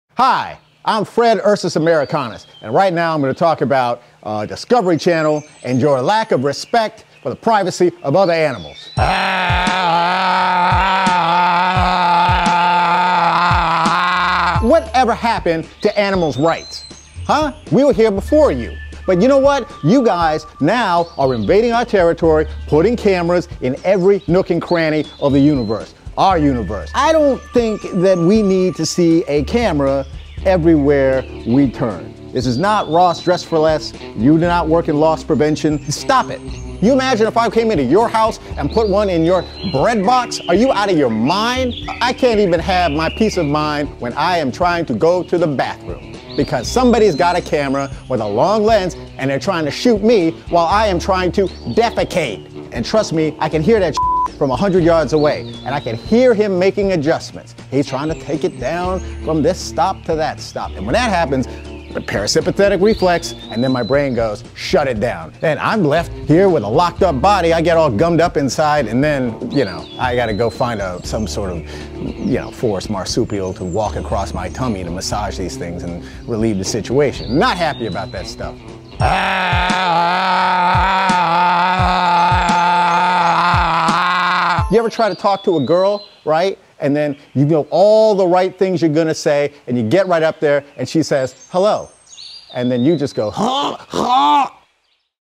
Bear Rants About Cameras Filming Nature | Heavy Petting Originals
Fred Ursus Americanus has some opinions about cameras filming nature.